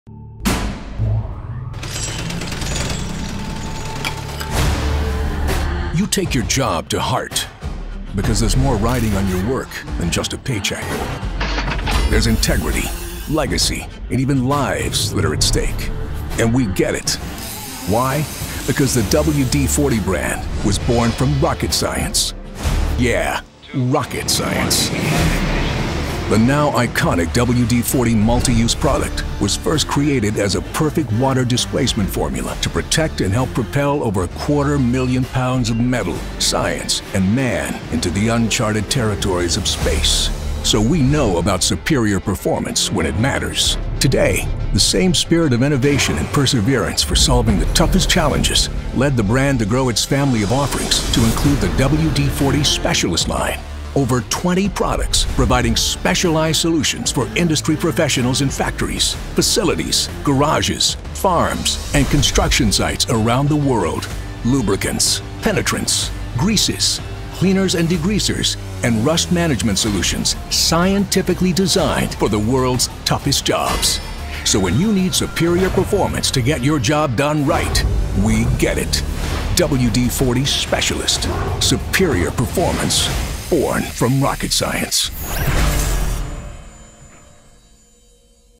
WD-40 Specialist®: Superior Performance, Born from Rocket Science™
With WD-40 Specialist®, for every challenge, there is a blue and yellow can. Almost 70 years ago, WD-40 Company scientists invented WD-40® Multi-Use Product, a water displacing formula that was key to sending rockets into space. Today, our scientific expertise has led us to create the WD-40 Specialist line of products of high-performing products that deliver superior performance for industry professionals.  WD-40 Specialist: best-in-class products for industry professionals.